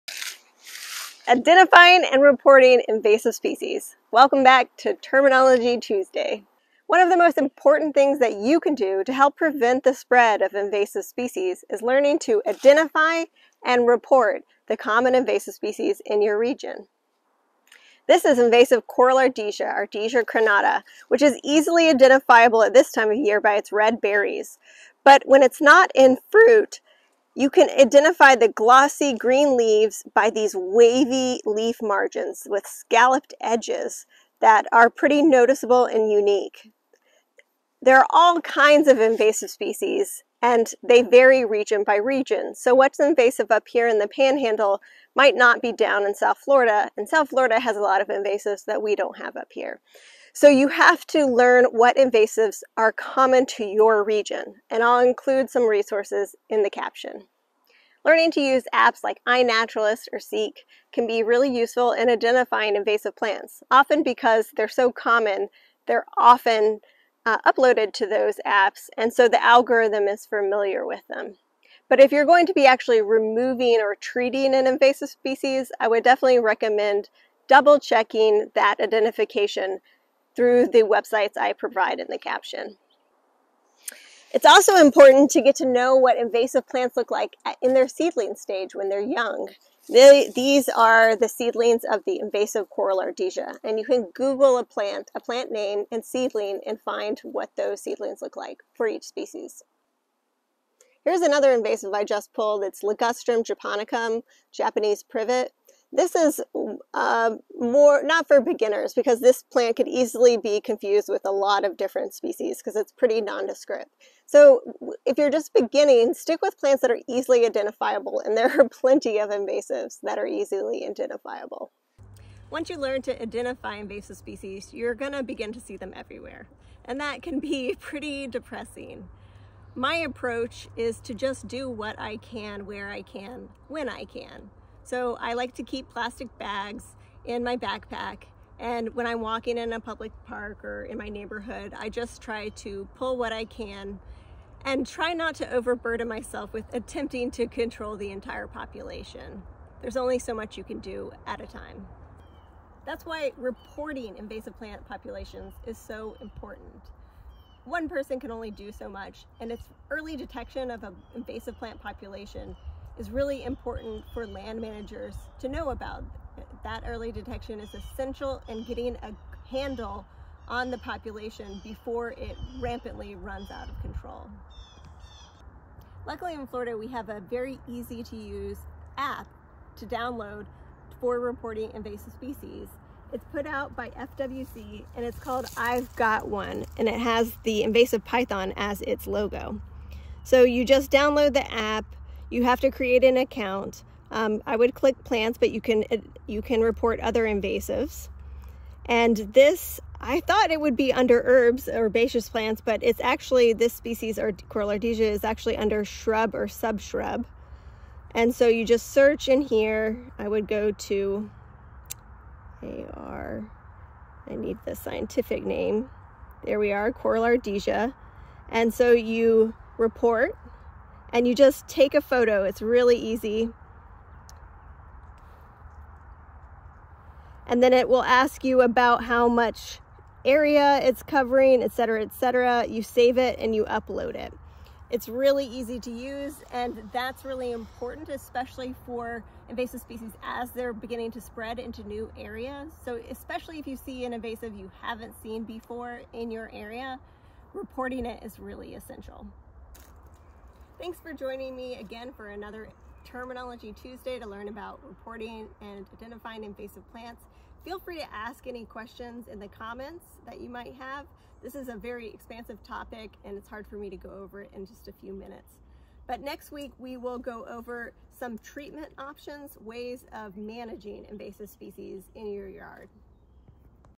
🌿 Identifying & Reporting Invasive Species
TERMINOLOGY TUESDAY 🌿
w/ the Florida Native Plant Society!
What can you do to help prevent the spread of invasive plants? Learn to identify and report invasive plants in your region! Use the links below to familiarize yourself with invasive plants in Florida. Some Florida counties provide lists of invasive species common to your area - google your county name and “invasive plants” to look for one.

Apps like INaturalist and Seek can also help you identify invasive plants in the field. Many invasives are commonly uploaded to these apps, so the algorithms can easily recognize them.

These apps can also be useful for reporting invasive plants, but it’s preferable to use the FWC/EDDMapS app, I’veGot1 (link below to website). This app aggregates data from other databases and organizations, as well as volunteer observations, to create a national network of invasive species and pest distribution data that is shared with educators, land managers, conservation biologists, and beyond. Early detection of invasive plants is critical to effective control.  Using this app to report invasives is easy and the easiest way to share your sightings with land managers.

* University of Florida’s Center for Aquatic and Invasive Plants
Plant Directory has photos, impact of the invasive species, and management options. It includes native plants in the directory, but they can be filtered out:

* Florida Natural Areas Inventory
Invasive Plant Directory with photos:

* Atlas of Florida Vascular Plants -
Enter scientific name of an invasive plant species to see a range map of Florida counties it is know to occur in: